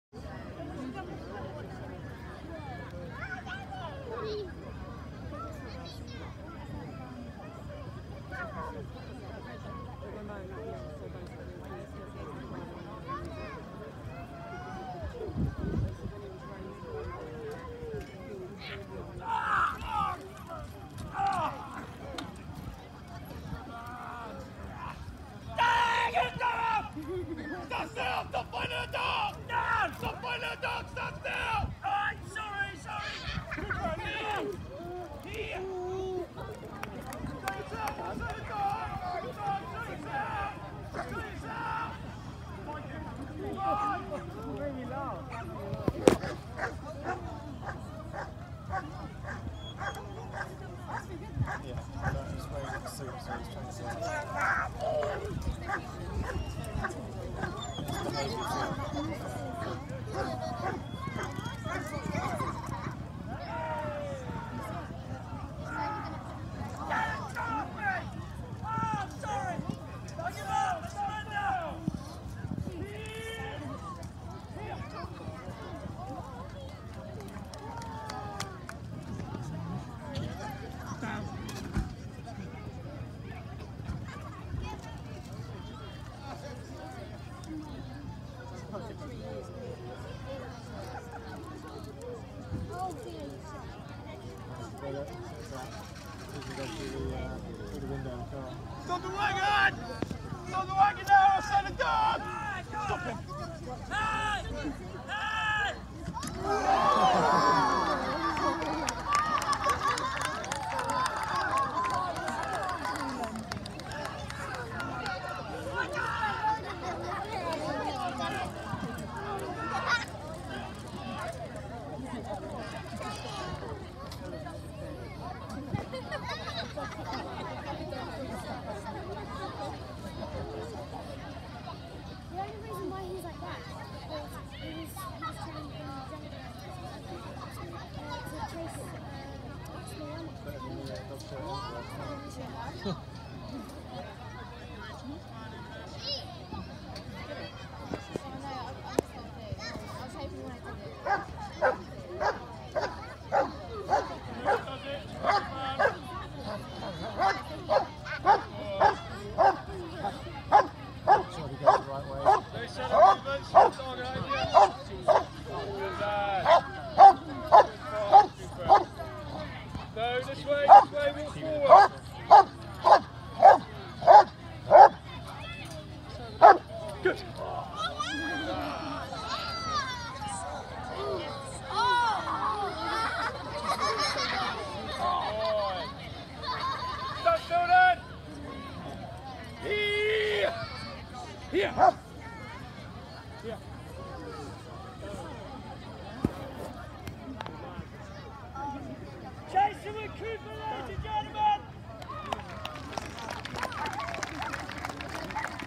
Family’s Day Display 2018
Armed forces family’s day Display with Jason Wilkey and Cooper, trained at Woodborough